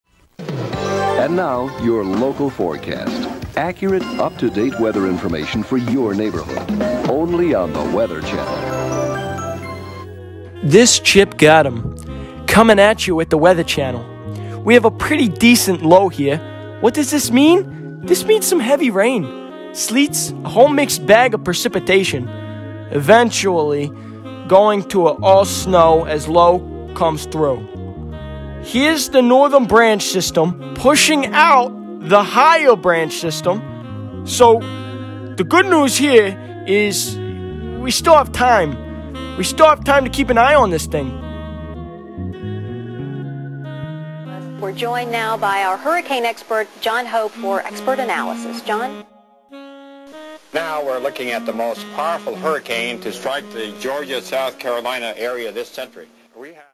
Chip Gotem live from the weather channel. 1992, March 12
a vintage weather report from the seattle area in march the 12 of 1992